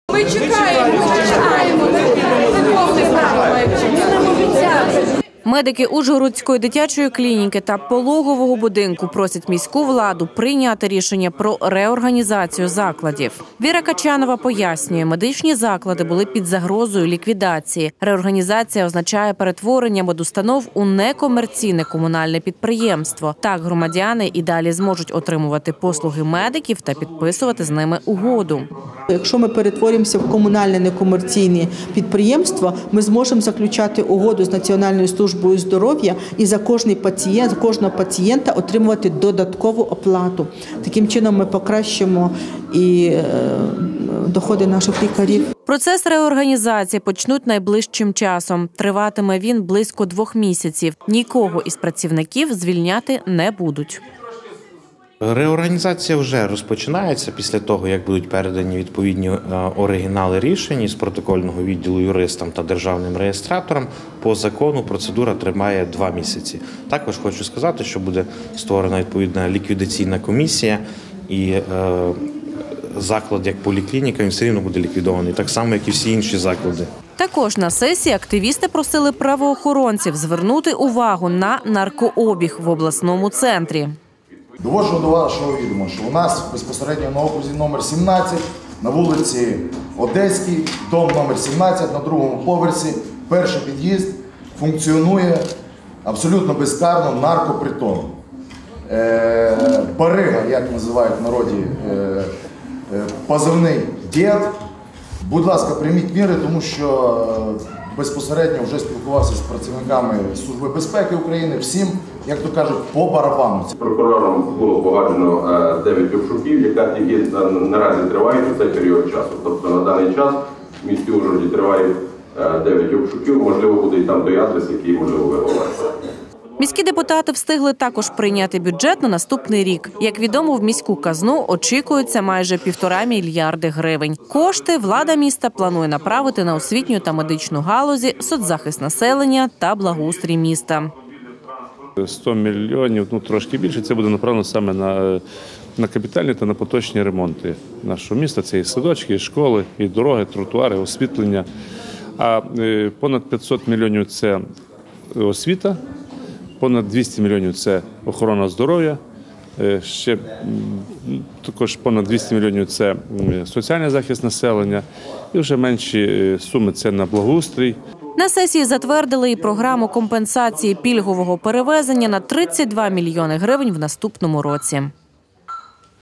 Сесія міської ради
В Ужгороді нині депутати міської ради приймали бюджет міста на наступний рік. На засіданні також депутати розглядали питання реорганізації міських медичних закладів. Самі медики прийшли та блокували трибуну з вимогою перевезти їх установи у некомерційне комунальне підприємство.